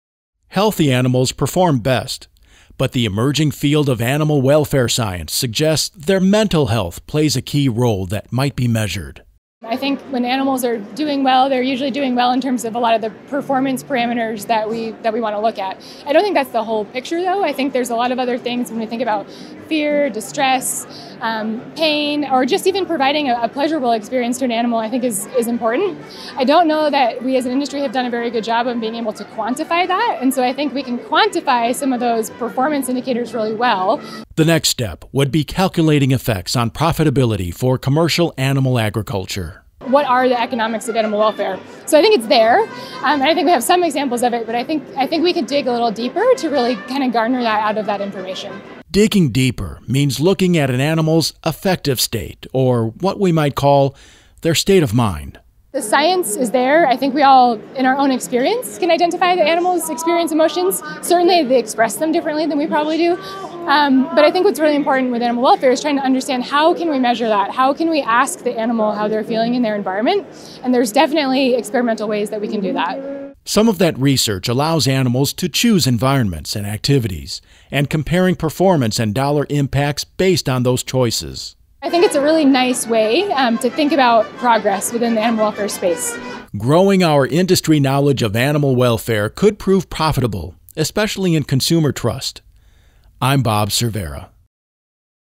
New lens on animal welfare
Lily Edwards-Callaway explains the evolvement of animal welfare science at the 2020 Cattle Industry Convention in San Antonio. This video news is provided by Certified Angus Beef LLC and the American Angus Association. for more information.